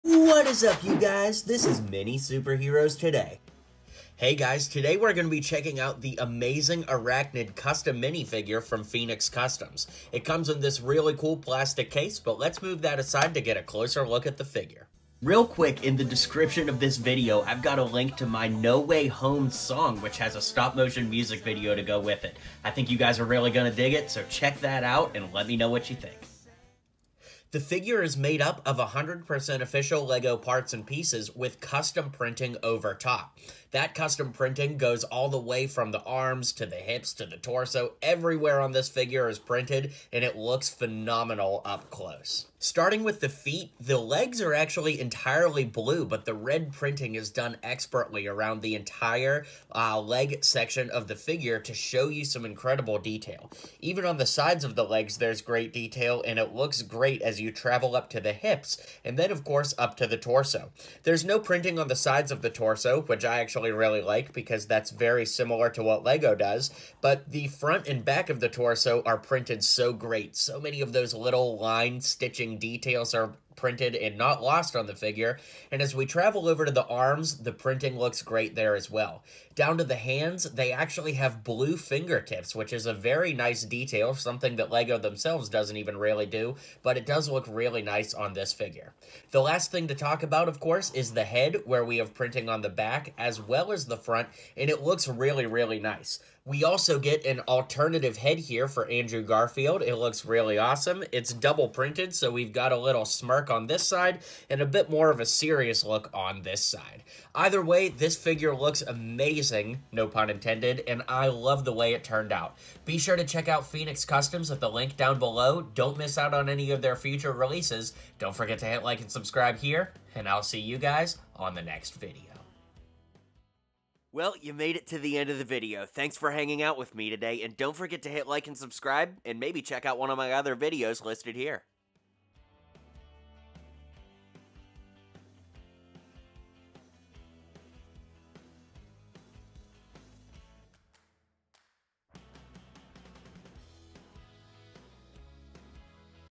AMAZING SPIDER-MAN Custom Minifigure - Phoenix Customs Review
Thanks to my friends over at Phoenix Customs, LLC for sending me this immaculate custom printed LEGO minifigure of Andrew Garfield's Amazing Spider-Man aka Amazing Arachnid! These are insanely detailed, and I am excited to share it with all of you guys!

What do you think of this figure? Drop a comment and let me know!